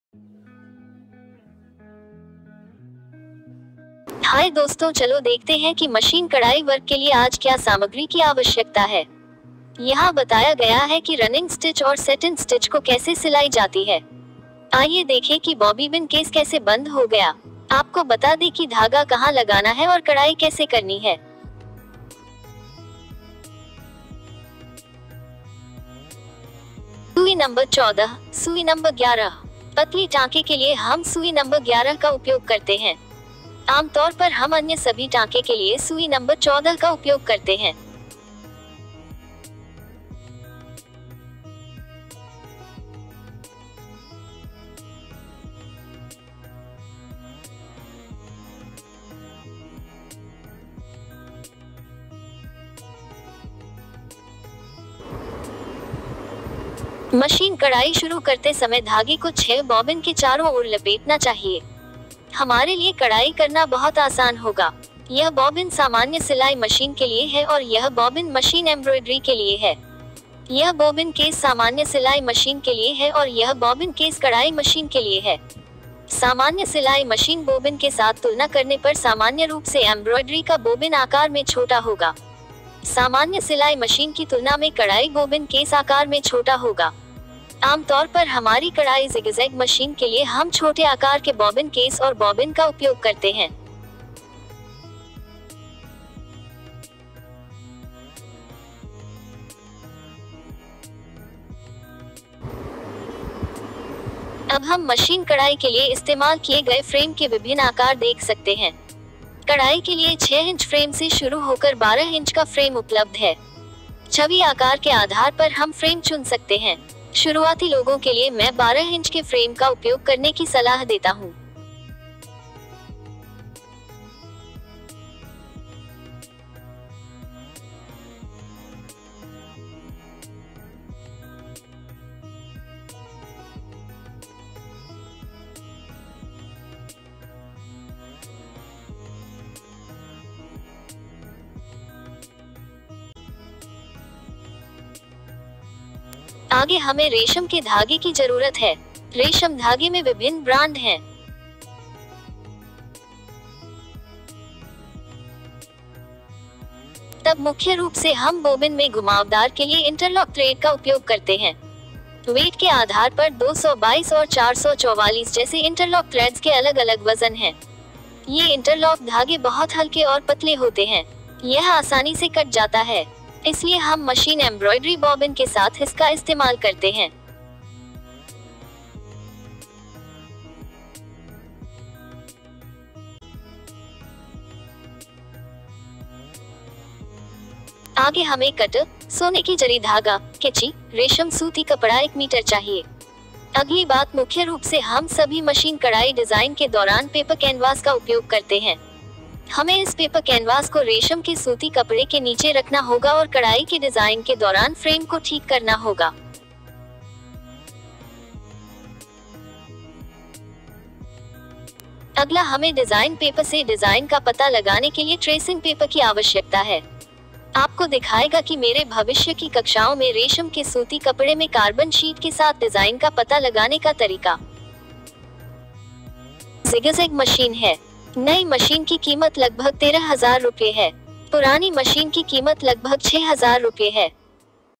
Machine Embroidery Class 2(Hindi) Materials Required|Machine Embroidery online class for beginners
Machine Embroidery Online class videos in sequence will be uploaded one by one. This will be helpful for all from the beginners, intermediate etc.

Please follow the instruction closely and you will be able to learn this of your own. Any clarifications or doubts please update in the comments. I will upload the clarification videos for your doubts in a day or two.

Further to my Machine Embroidery classes planning to upload the videos for Aari work, Kuchu Work, Hand Embroidery  and lot more.